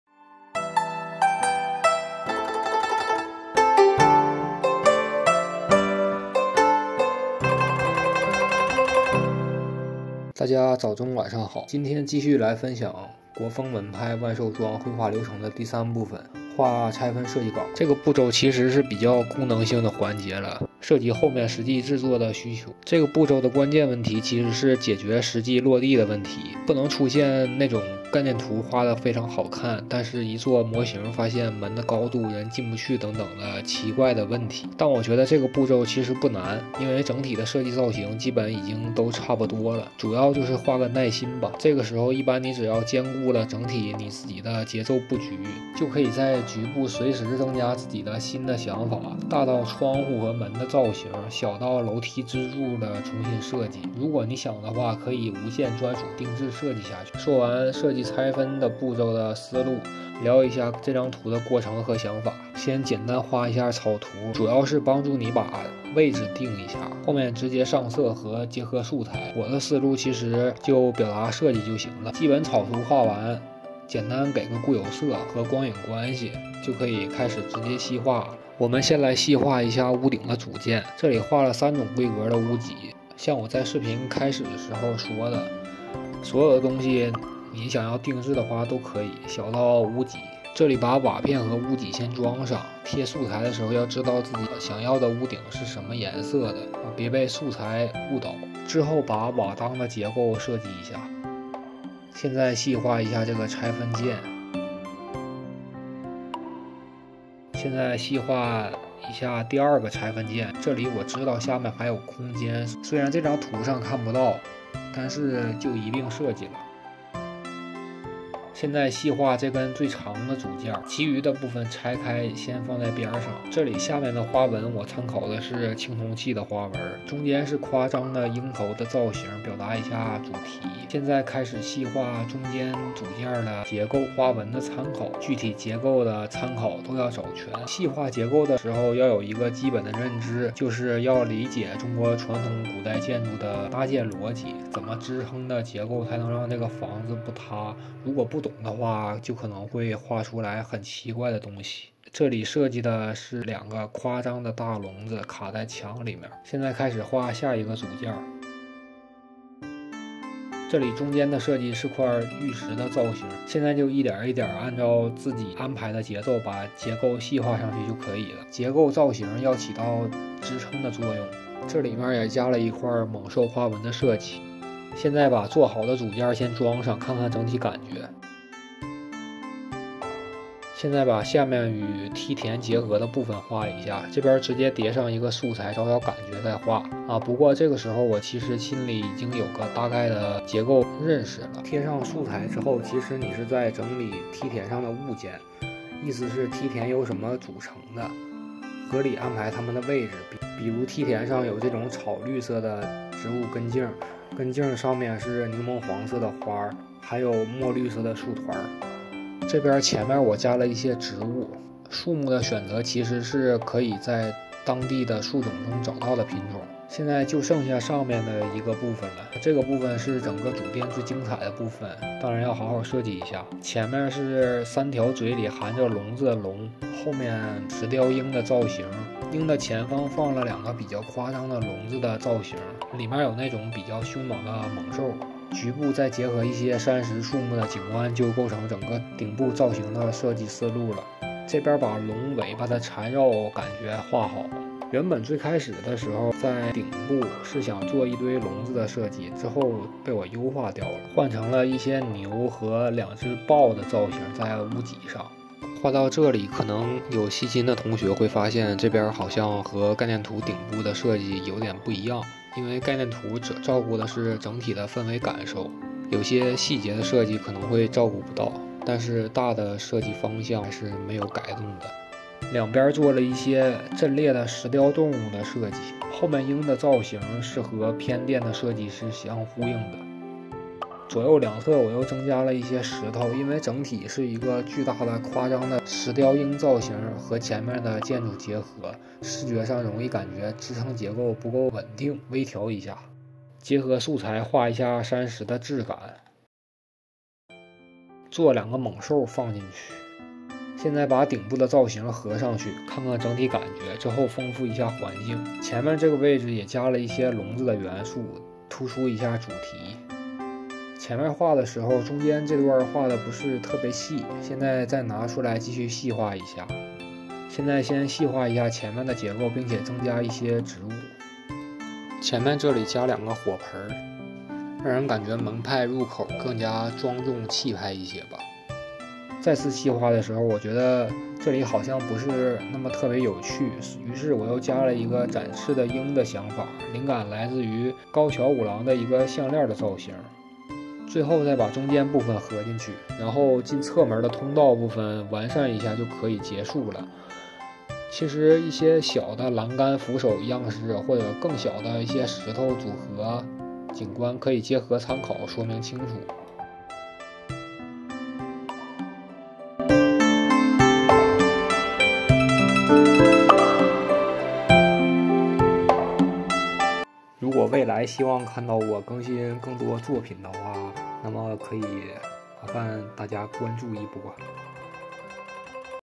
Sect of Chinese hunter , painting process3 , environment concept art of video game 万兽庄概念图3
This is a painting process to share with u of my thoughts.This is my latest art work  which is very time-consuming.The theme of this series depicks a Chinese sect.People in this sect hunt and domesticate animals for living.If u like my art work,please subscribe my channel.